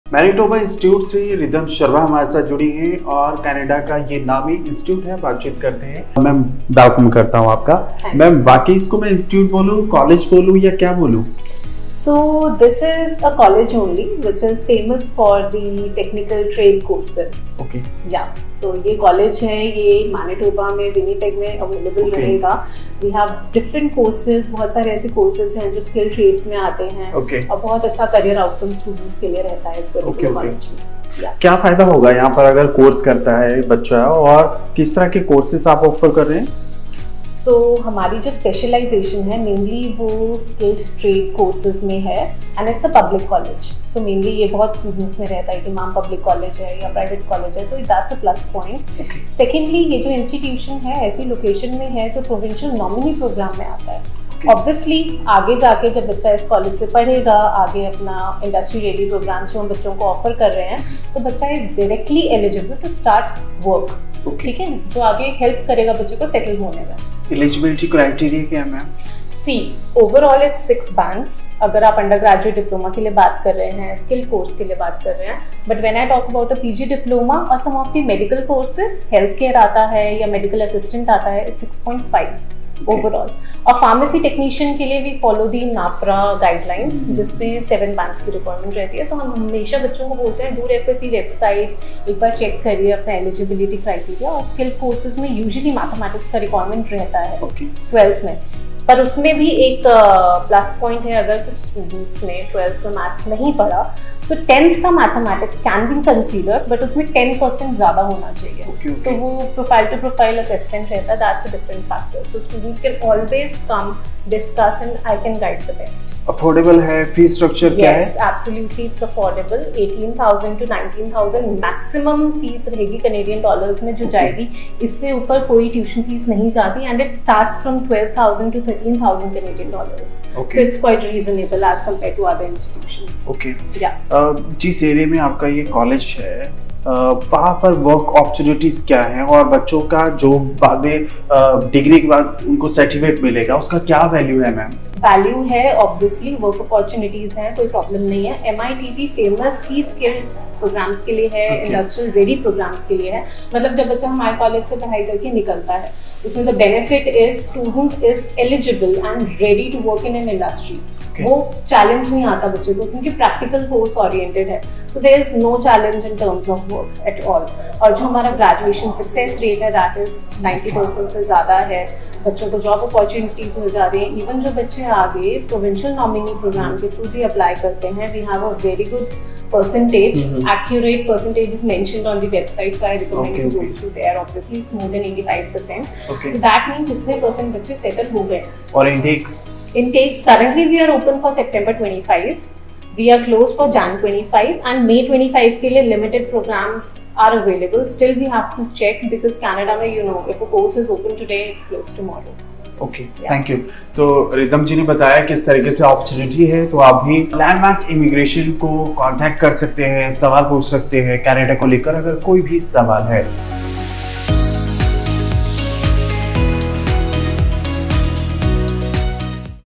Study in Canada at MITT | Affordable Education & Career Opportunities in Winnipeg! 🇨🇦
🎓 Study in Canada at the prestigious Manitoba Institute of Trades and Technology (MITT) in Winnipeg! 🇨🇦

With affordable fees starting at 12,000 CAD, MITT is a public college specializing in technical and trade courses in a prime PNP location.

Unlock your future with practical skills and career opportunities! Apply now through Landmark Immigration.